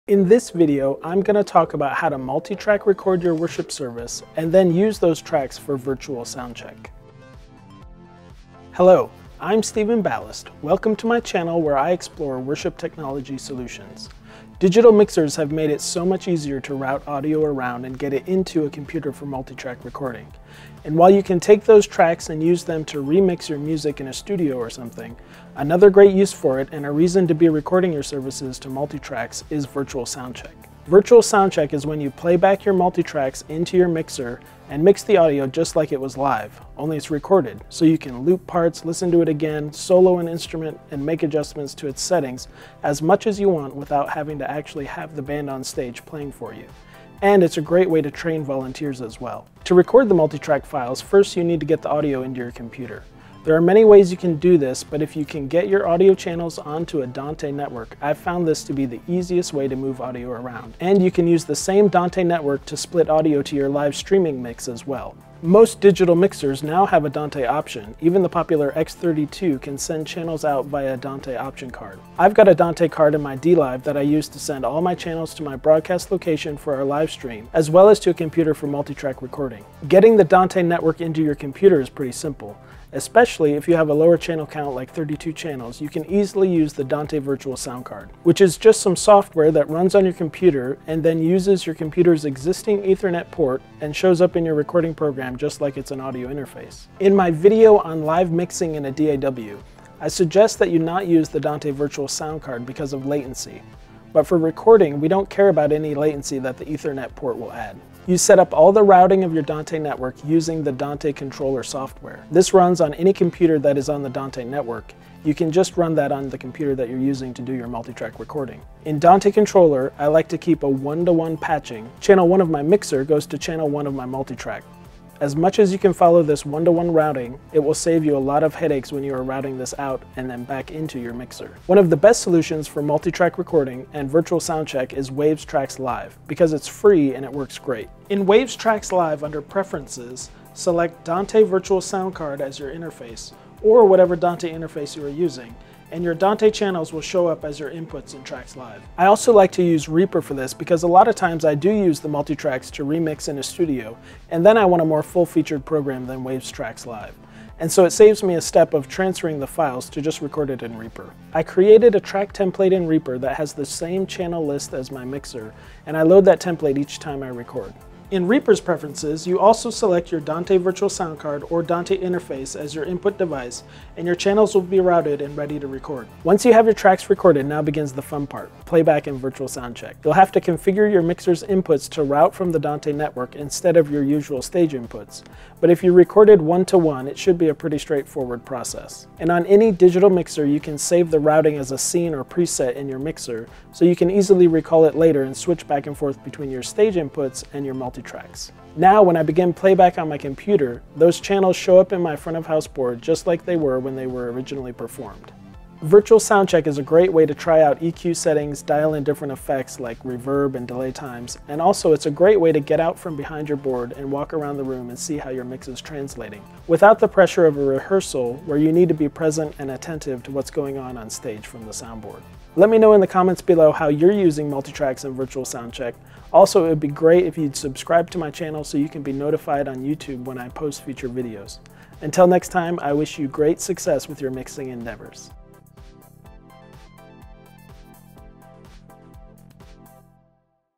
Virtual Sound Check with Live Multitrack Recording - Waves Tracks Live or Reaper
In this video I discuss how to multitrack record your live worship services, and then use those tracks for virtual sound check.  You can use Waves Tracks Live, or Reaper as cheap, effective ways to record and playback your multitracks.

The network switch I recommend for Dante is:
Cisco SG-300

x-Dante Card for X-32 Mixer